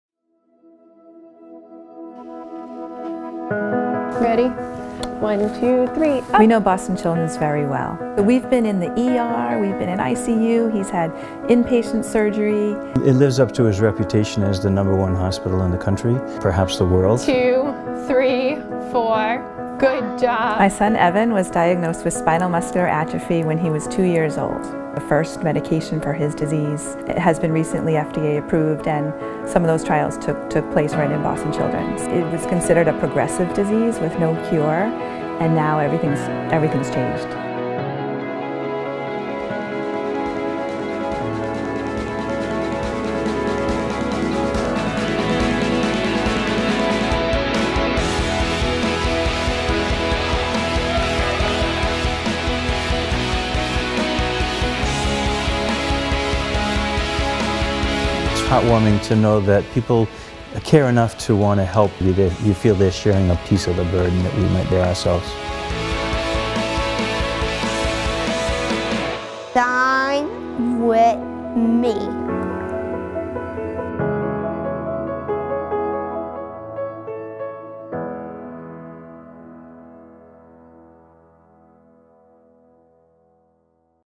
Shine your light for the kids of Boston Children's Hospital
This February, celebrate lifesaving work. Kids like Evan count on Boston Children’s to bring them brighter, healthier days. Donate $10 or more and receive your shining wristband—a symbol of your support. All funds raised will fuel research and care at Boston Children’s, the nation’s #1 pediatric hospital.